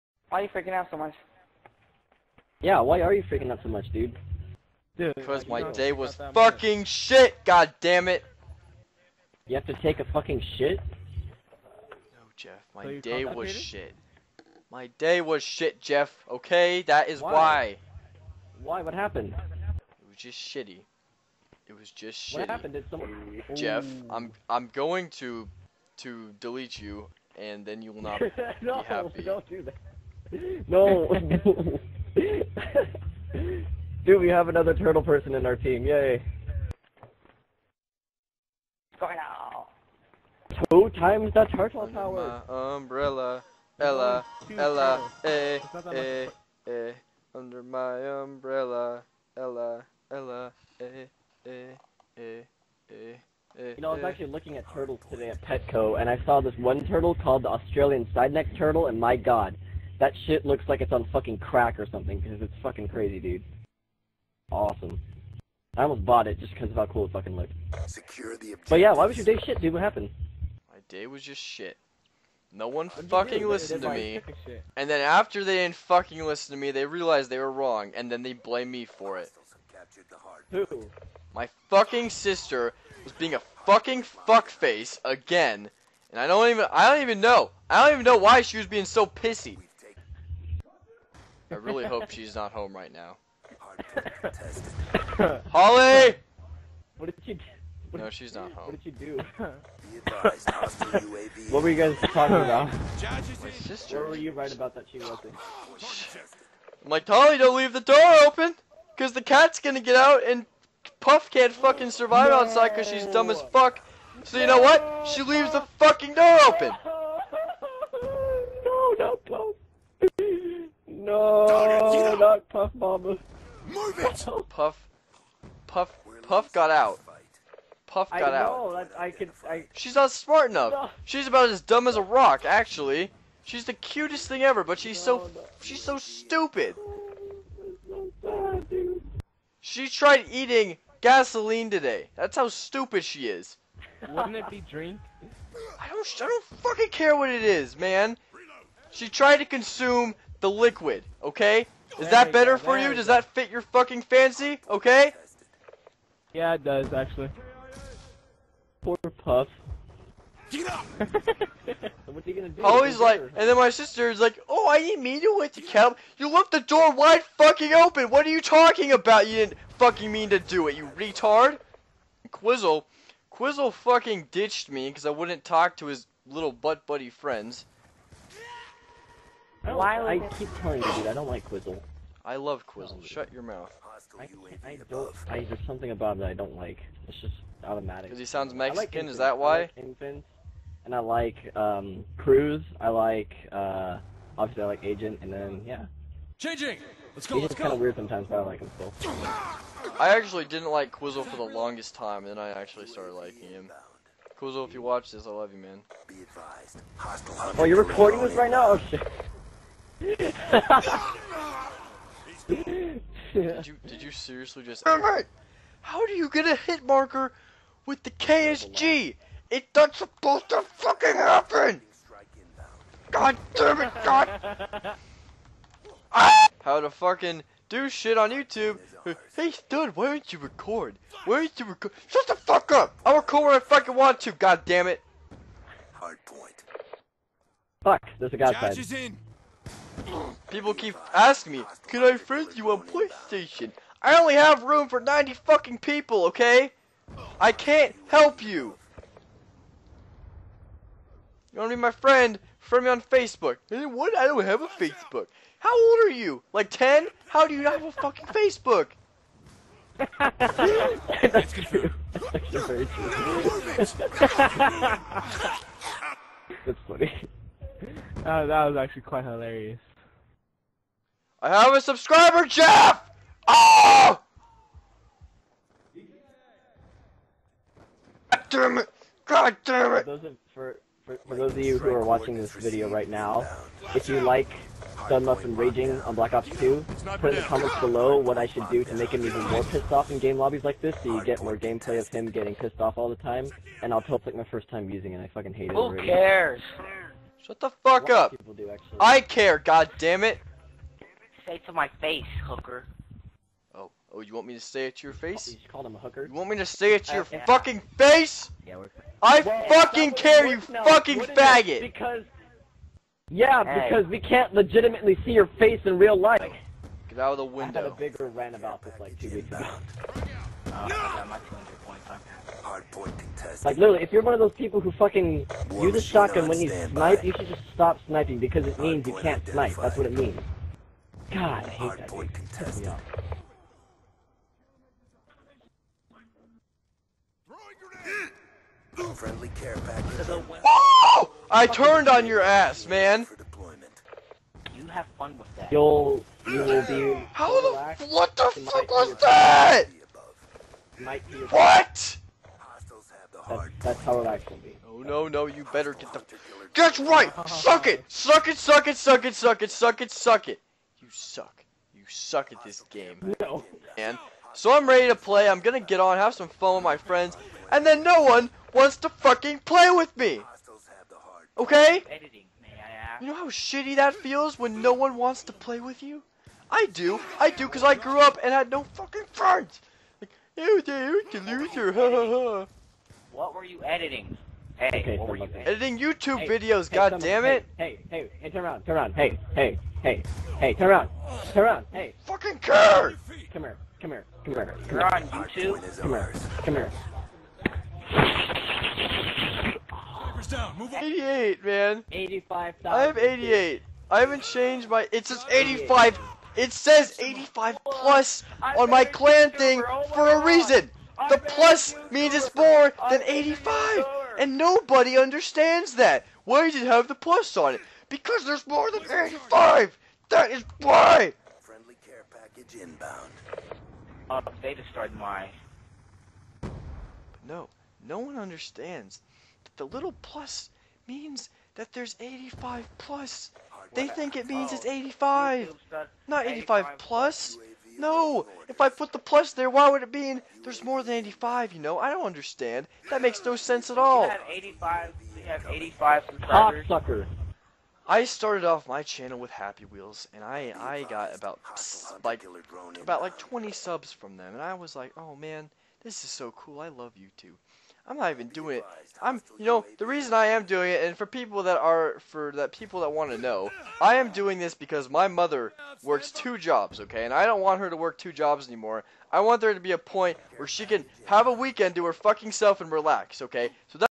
Youtube Truth Freakout! **READ DESCRIPTION**
So hello everyone in this video I was in a lot of stress and I just had to let it all out im not usually like this. I was trying to let it all out and for some reason I was recording so don't think of